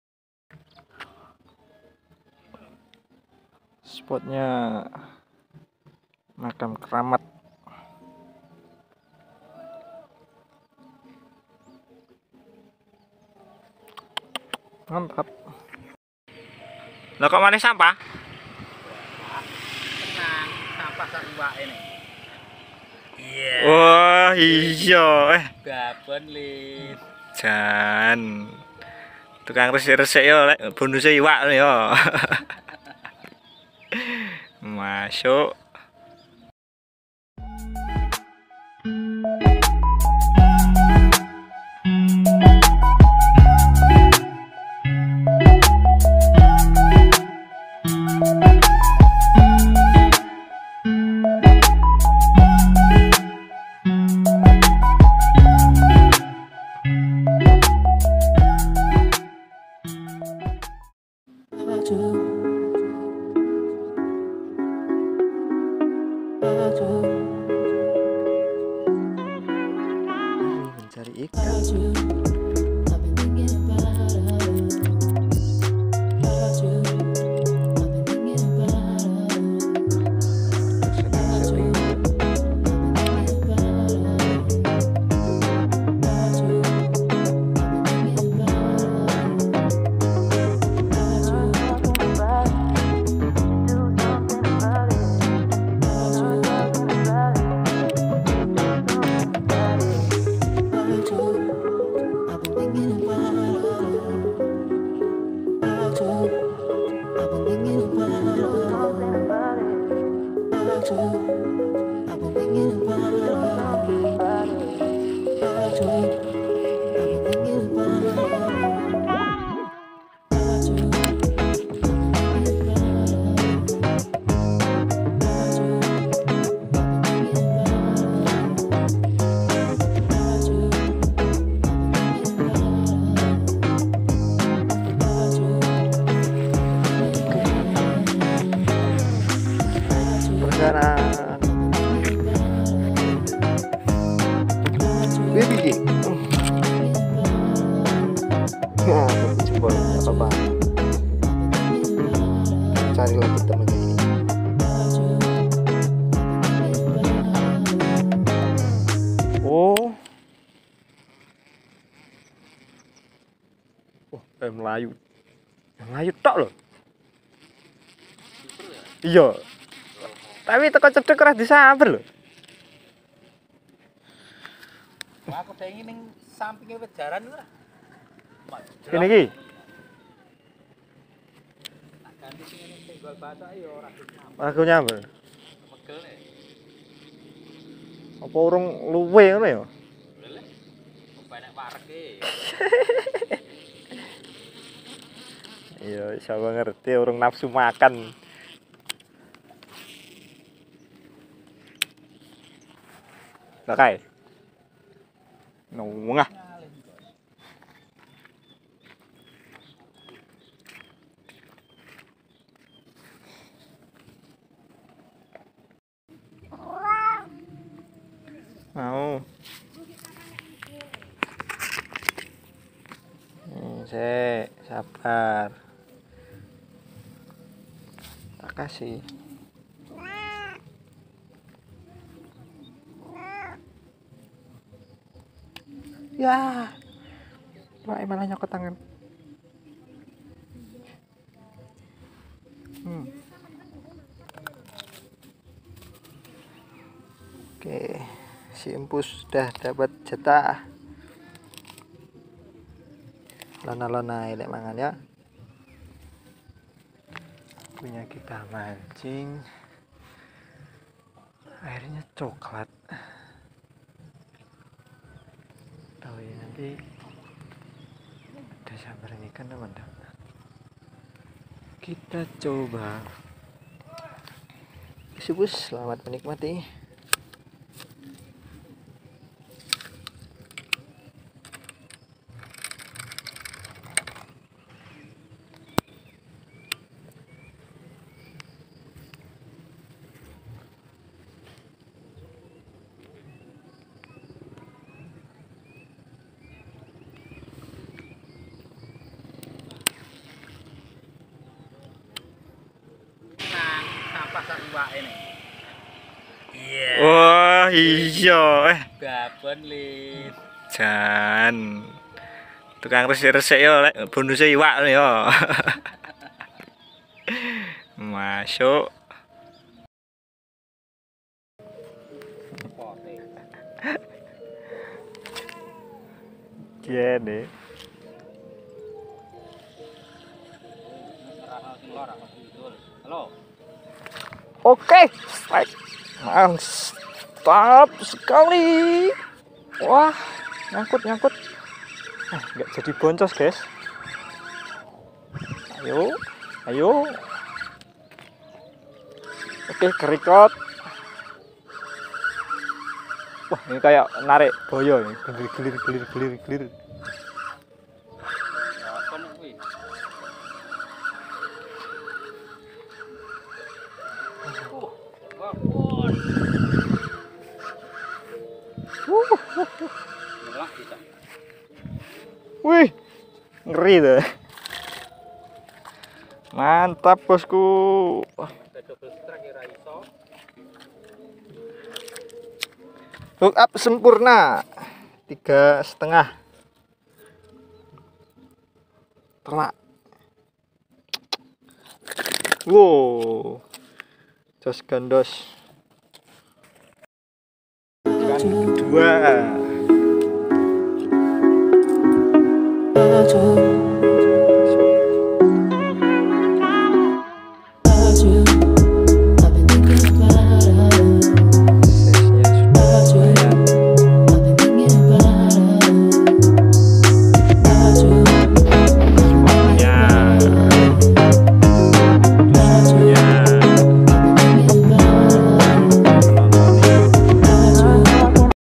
MANCING IKAN GABUS ||CASTING makam tenggelam
keseruan mancing tehnik casting menggunakan rood custom BC dan spining di spot rawa-rawa yang terbentuk dari curah hujan tinggi di kota ponorogo